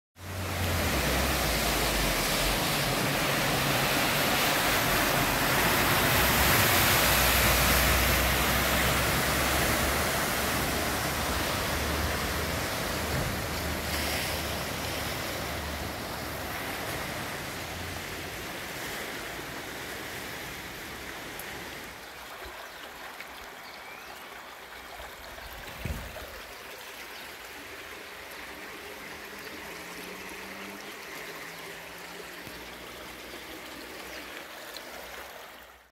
Inundaciones en Torrevieja (20/05/2023)
¡Calles como ríos! La fuerte tormenta de esta tarde ha descargado en algunas zonas de Torrevieja hasta 20 litros por metro cuadrado en menos de una hora.

En algunas zonas de la ciudad se ha llegado a acumular bastante agua en las calles. Una de ellas es la zona de Doña Inés donde la calle parecía un río.

Las imágenes nos las envía Marzena Kiebasz.